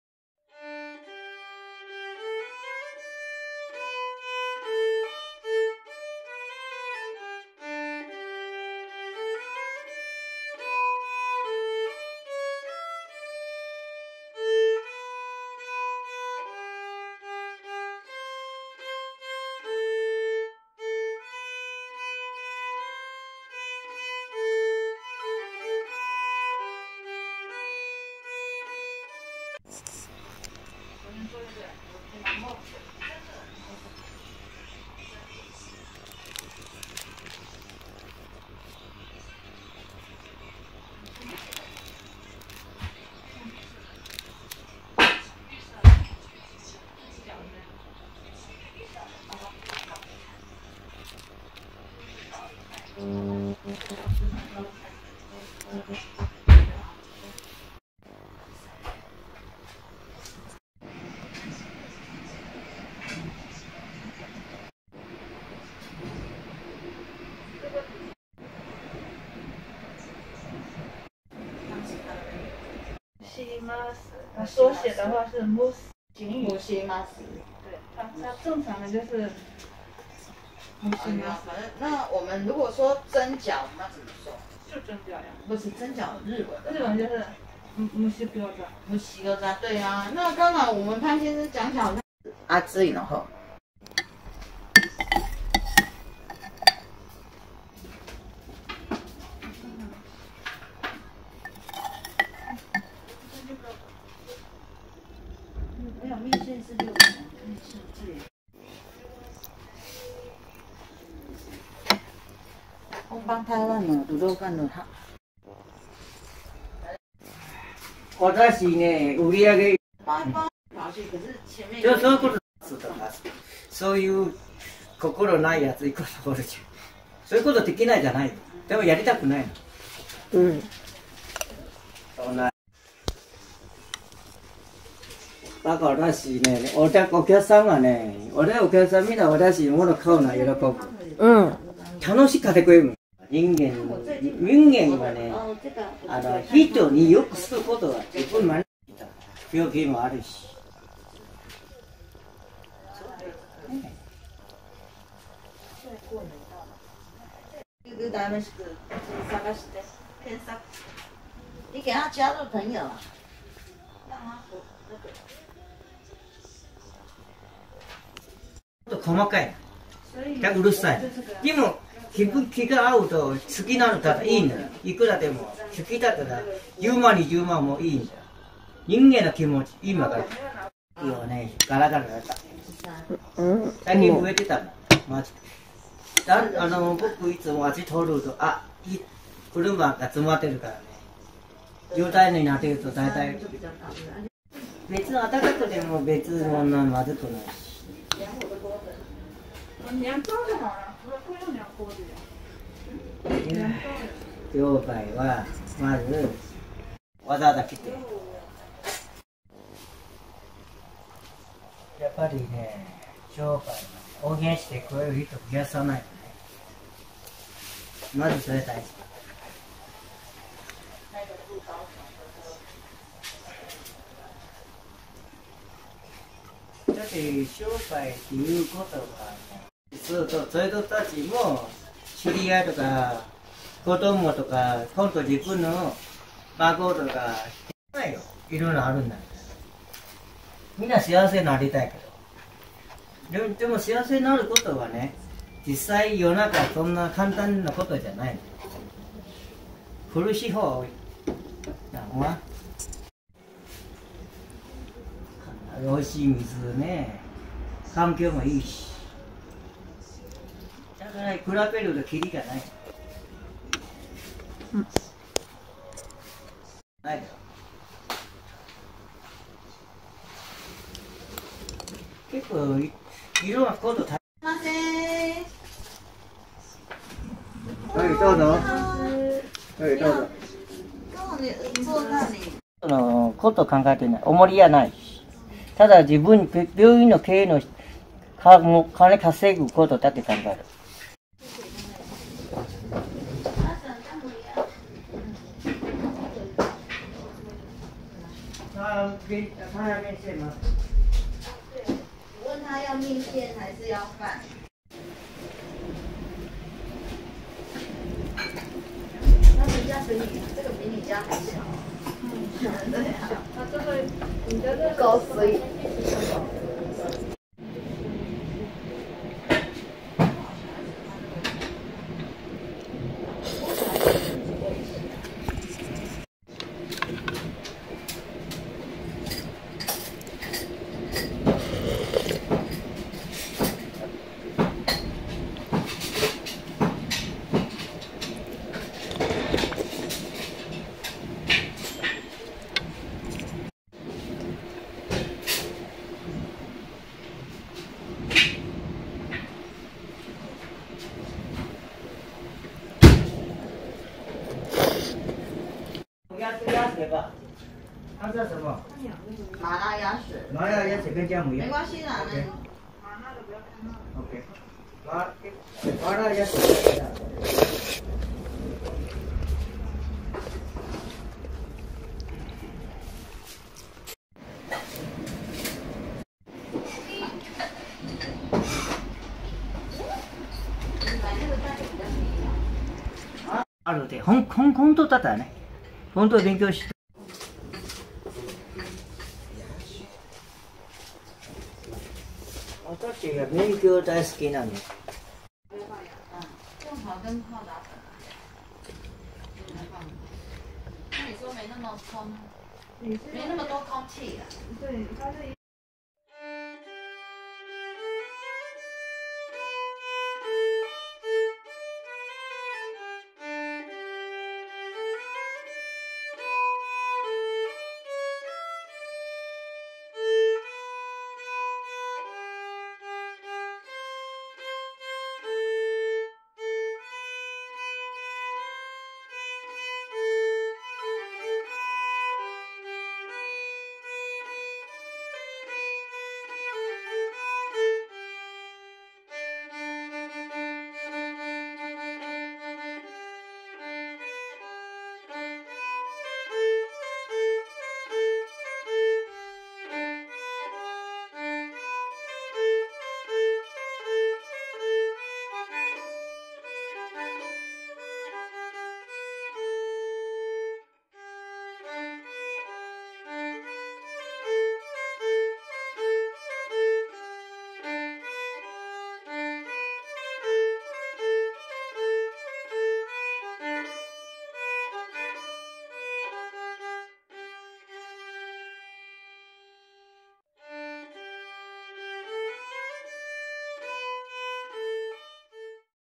【台湾屋台料理】 4K 大腸麺線 ルーロー飯 蒸し餃子 ホットタピオカミルクティー QQ台湾美食 with Sony XPERIA1 Ⅱ
QQ台灣美食 名古屋市港区

大腸麺線が餡だくで美味!!!
友人に教えてもらったホットタピオカミルクティーがまた美味しいんです
台湾人の潘さんによると、台湾人でもそうは飲まない珍しいドリンクみたい
だって台湾は寒い時期が短いですからね～
そして蒸し餃子
焼きも美味しいんですが、餃子のうまみが凝縮しててこれはハマる!!!
お薦めです!

撮影使用端末 Sony Sony XPERIA1 Ⅱ Cinema Pro
音楽編集ソフト Audacity
演奏 自分